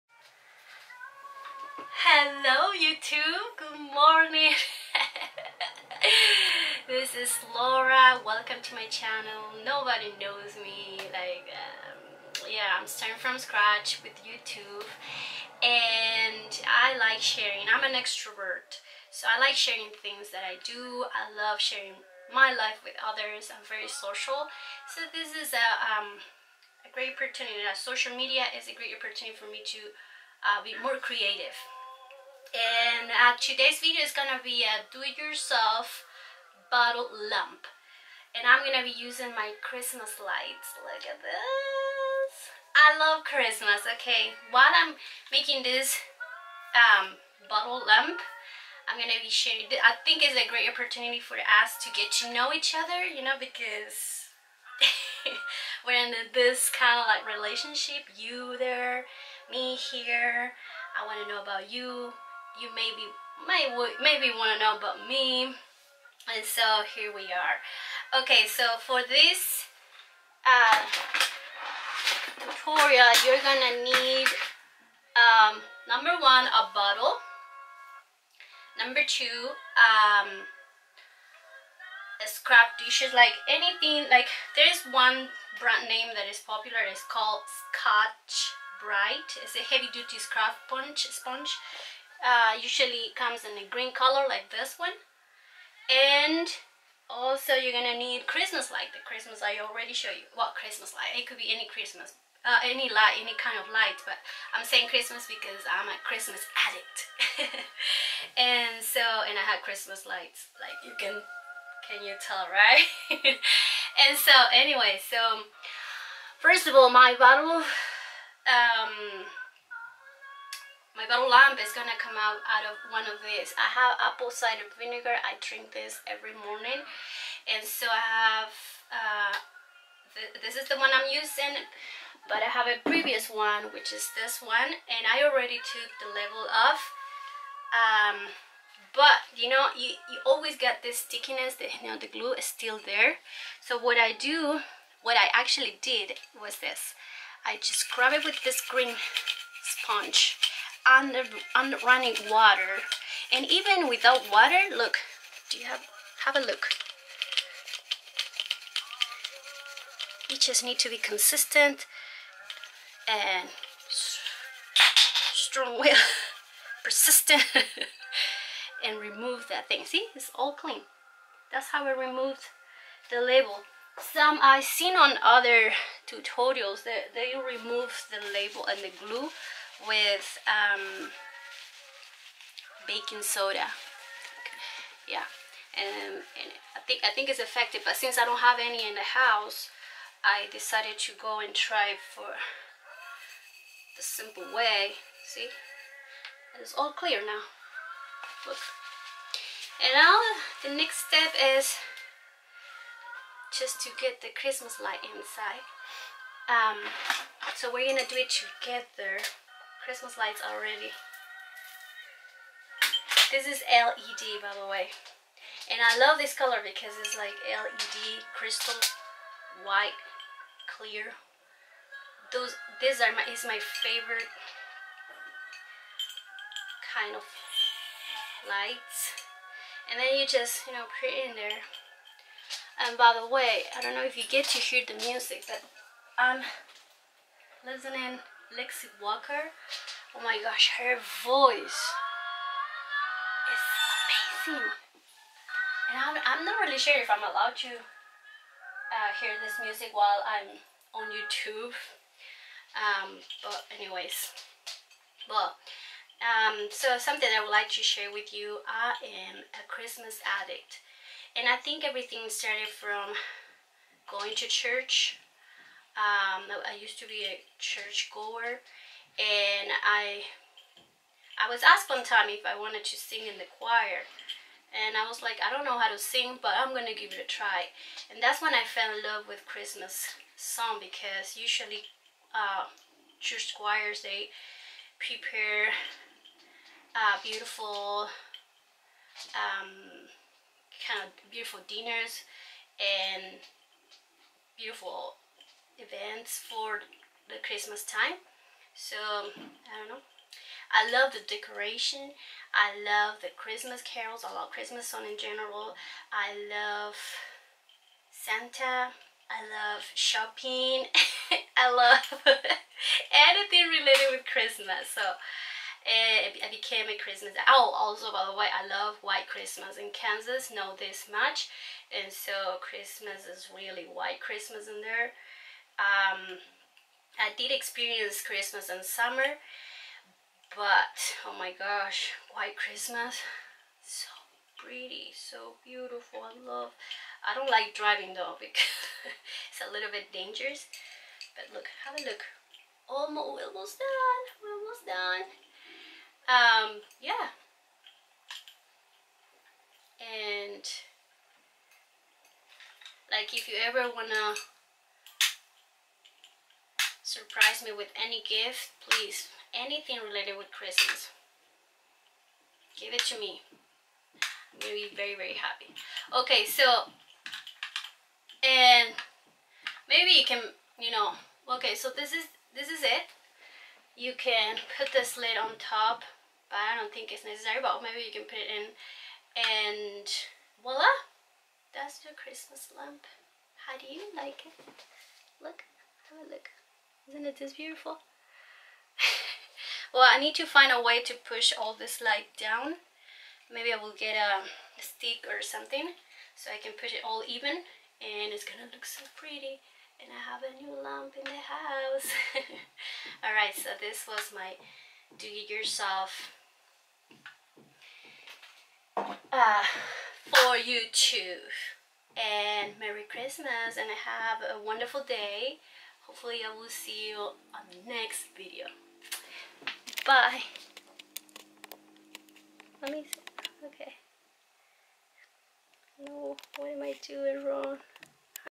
DIY Bottle Lamp (Christmas Led Lights)
My very easy tutorial on how to make a Bottle Lamp with Christmas led lights. You are going to need 1 Empty bottle, 2. The scotch brite aka scrub sponge to remove the label/glue if any. and 3. Christmas LED crystal white lights or any you prefer.